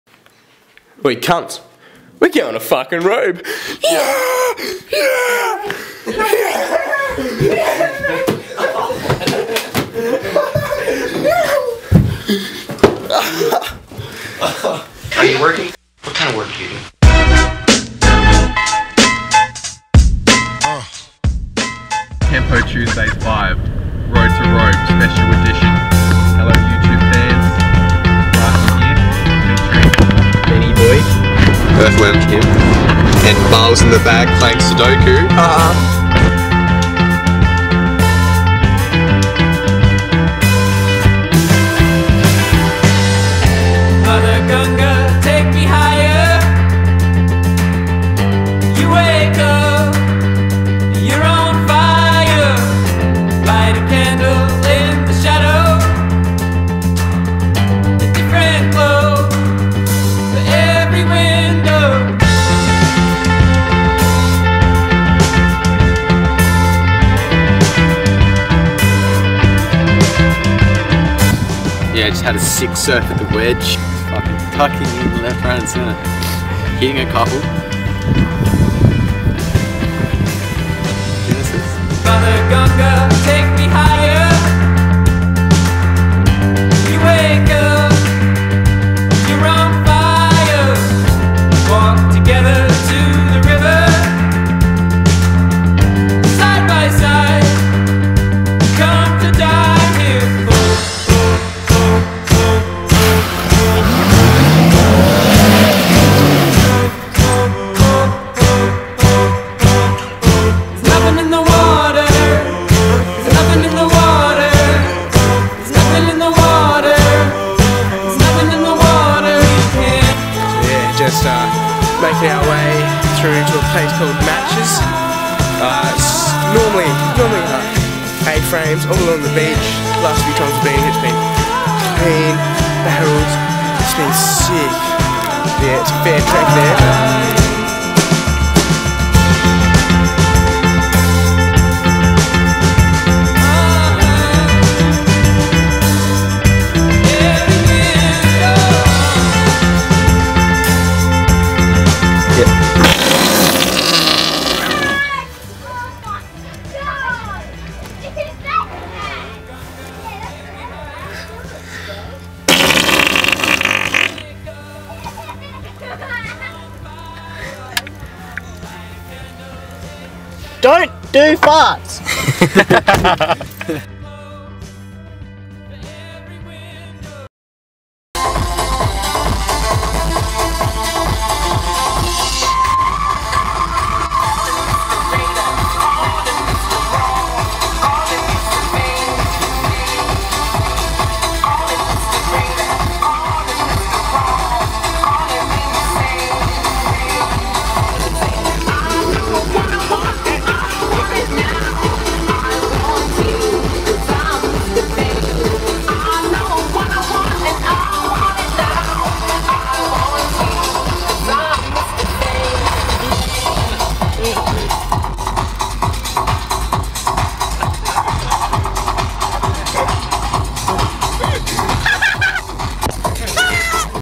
Tuesdays with Tempo # 5 Road to Robe
3 members of Tempo venture on a journey to the east coasr robe.. some say robes not famous for robes and there not wrong, there most famous for the robe easter classic a contest that has been running for 40 yrs.. but we say fuck that were free surfing. surf wasnt the best this year but we still had a blast